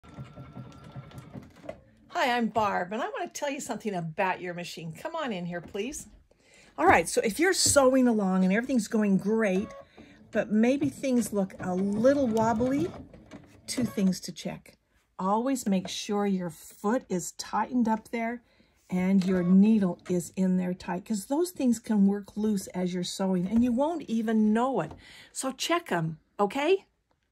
Tighten those foot and needle screws!
Keep your presser foot and needle solid by checking the screws.  Keep them tightened!
Join me for classes at @Sewwithbarb.com